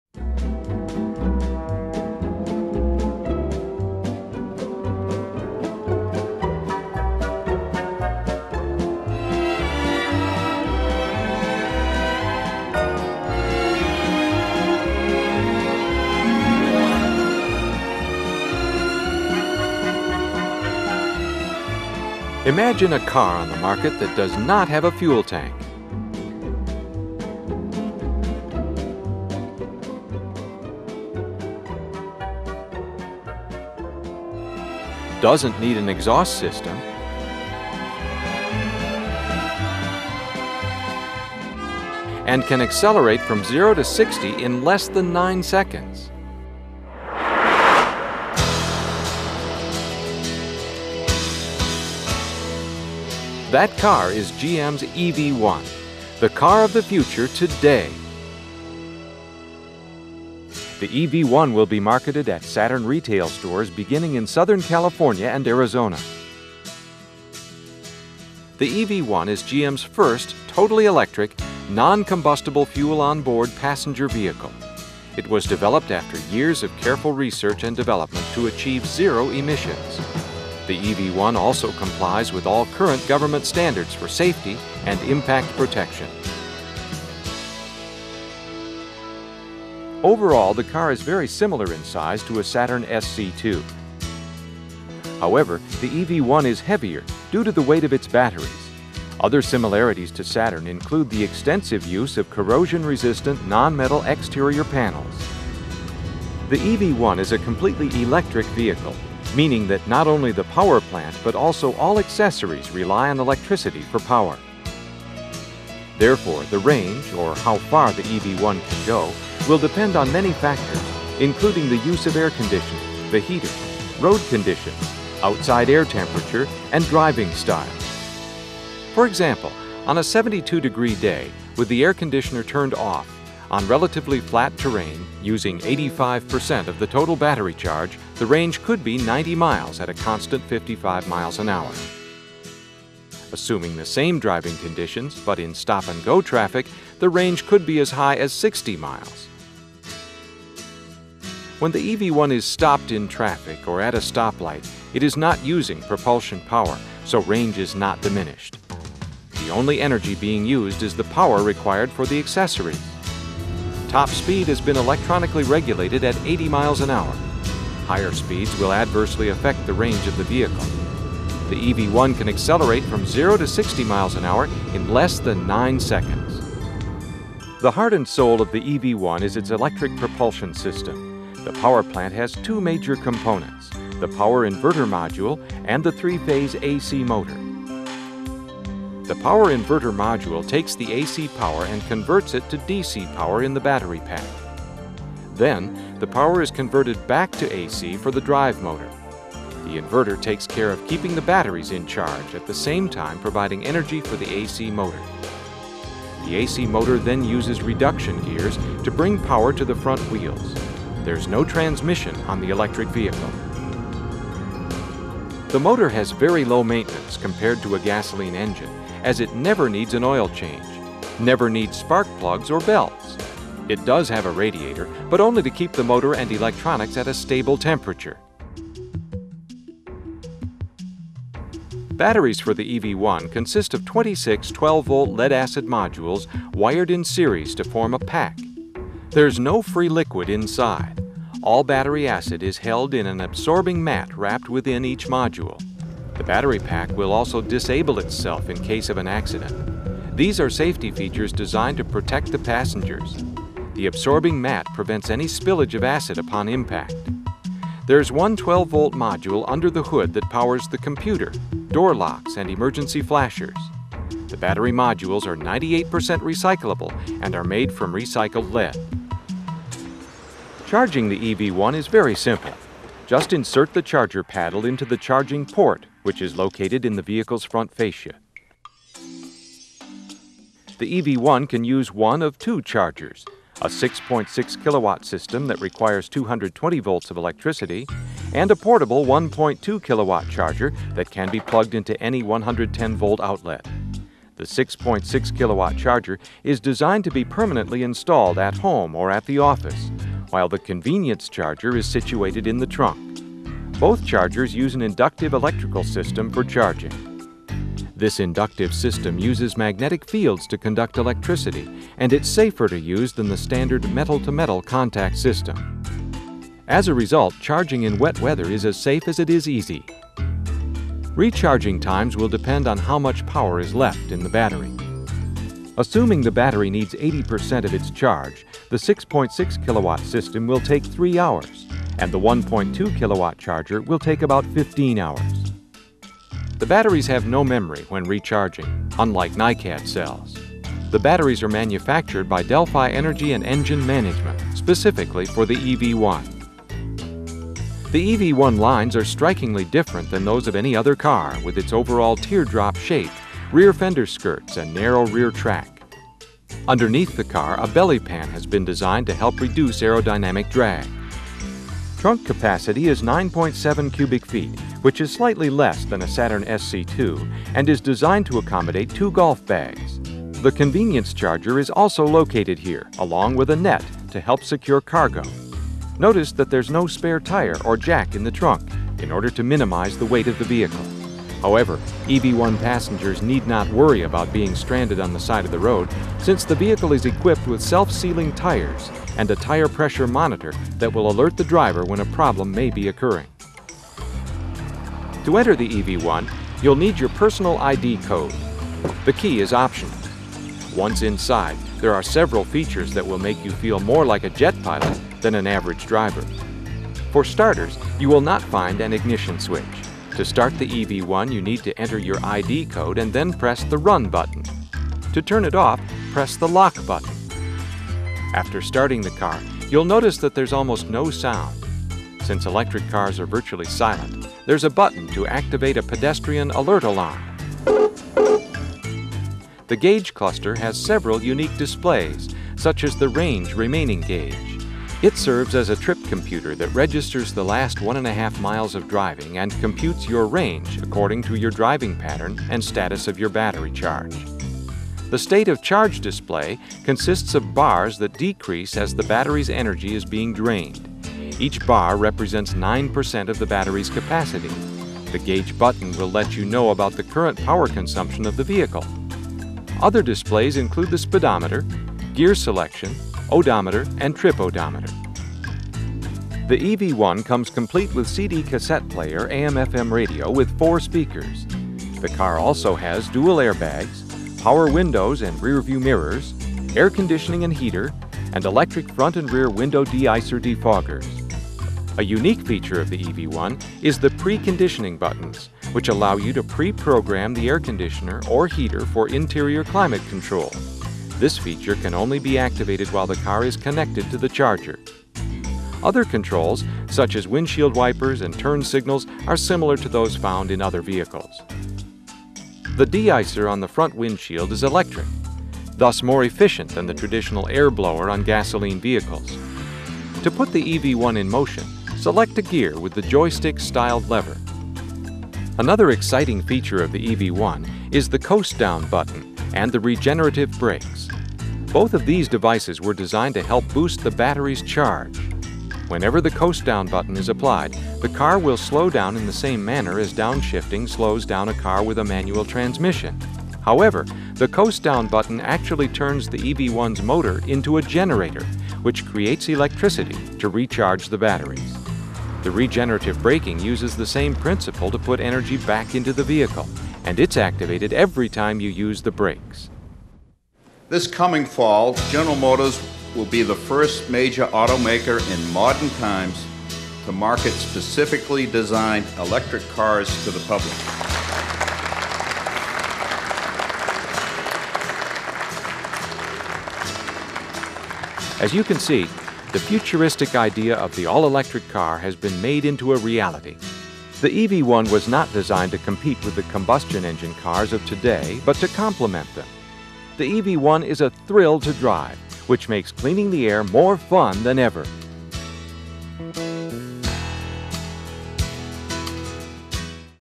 Blast From the Past: A look back at the EV1
A decade ago GM's electric car, the EV1, was a flop in the marketplace. But with gas over four dollars a gallon it would probably attract a lot more attention today.

Back when it was launched, the EV1 was revolutionary. It had a host of innovative features like regenerative braking and light-weight construction. But none of that could stop GM from pulling the plug on the program just a few years ago.

Take a look at this GM corporate video from the Autoline archives which highlights many of the EV1's features and shows how relevant the car is today.